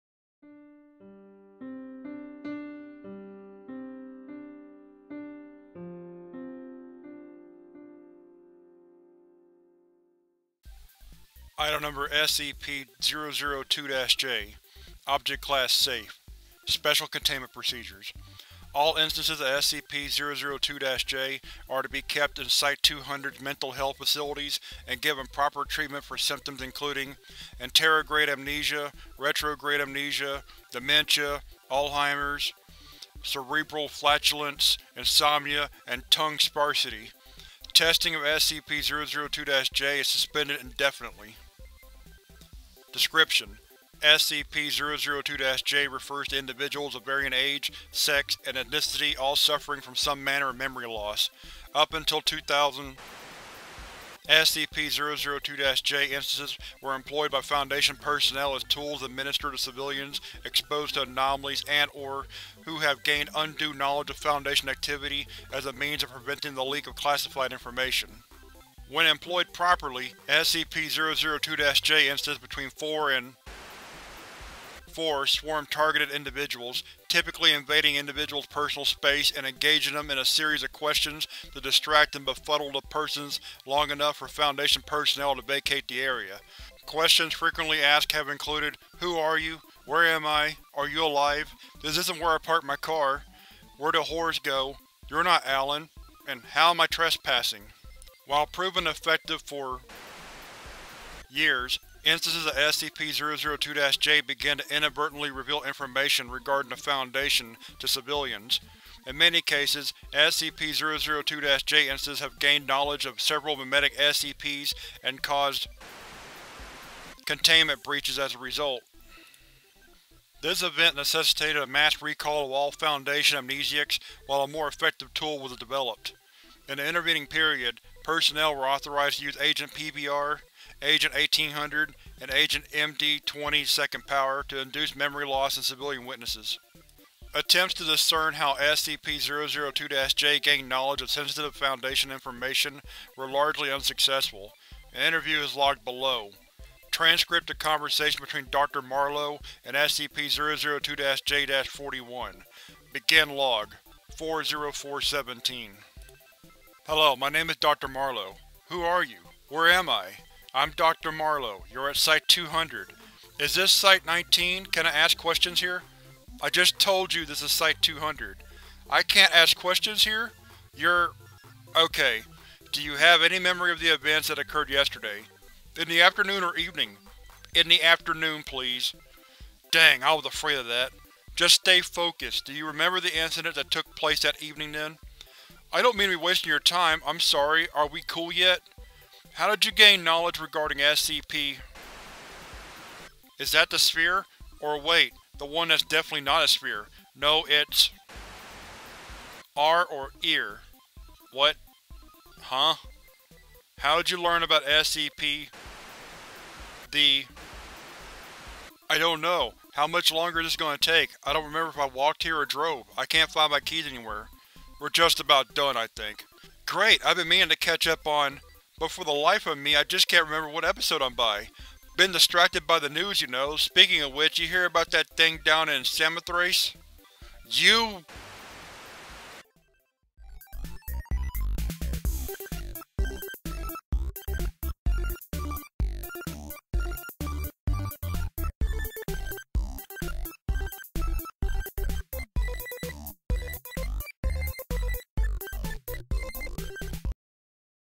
SCP-002-J Amnesiac Treatment | Joke SCP
SCP-002-J refers to individuals of varying age, sex, and ethnicity all suffering from some manner of memory loss. Up until 20██, SCP-002-J instances were employed by Foundation personnel as tools administered to civilians exposed to anomalies and/or who have gained undue knowledge of Foundation activity as a means of preventing the leak of classified information.